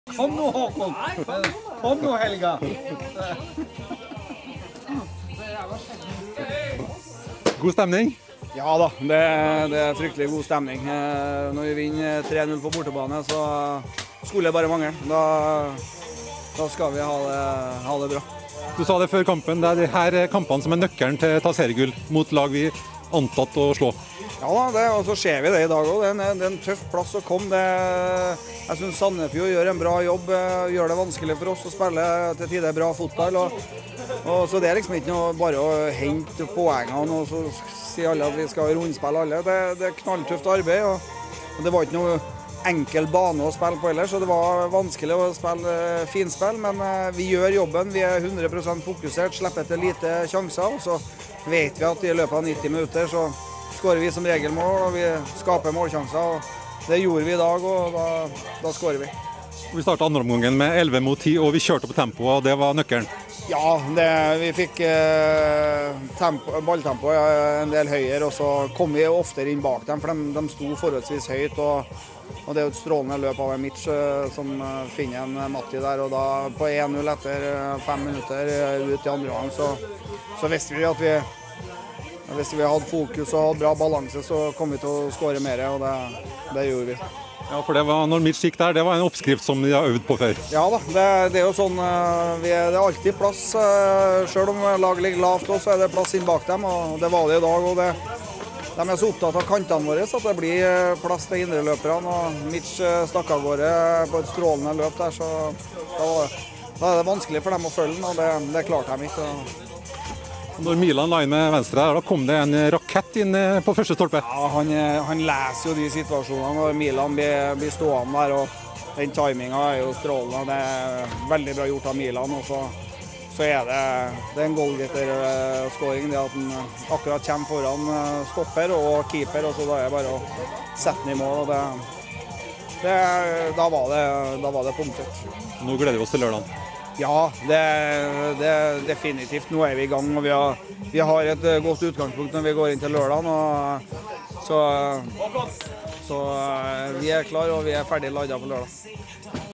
- Vi skal være fulladet til lørdag
SANDEFJORD: Kåre Ingebrigtsen har nå rettet blikket mot storkampen mot Molde på lørdag